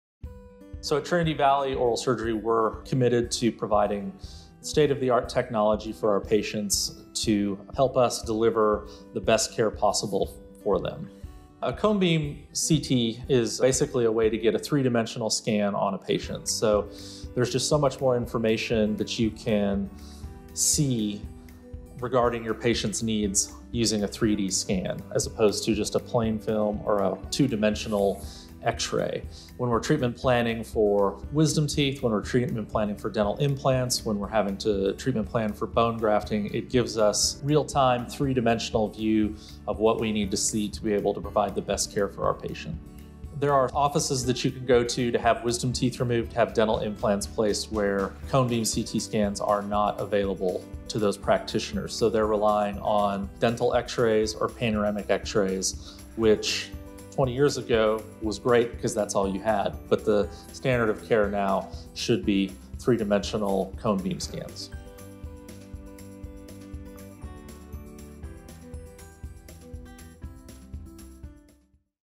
Dr. Reed Gibbins | Our Office Technology
Dr. Gibbins talks about the state-of-the-art technology we have in our office, specifically the CBCT and how it allows for better planning and treatment for our patients.